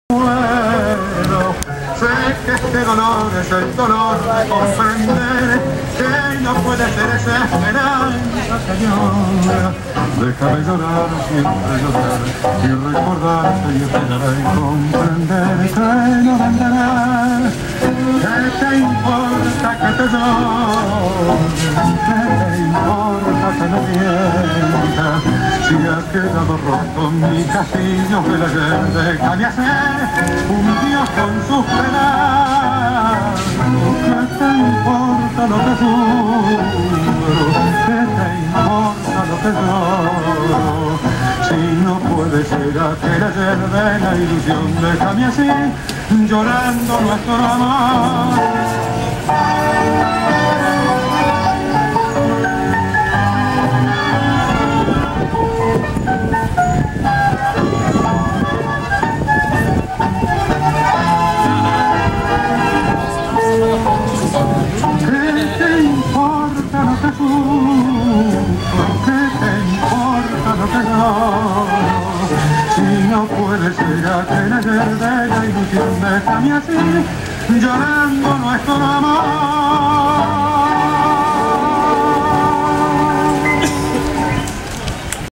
Tango Domingo
Tango in the park of Recoleta Buenos Aires, Argentina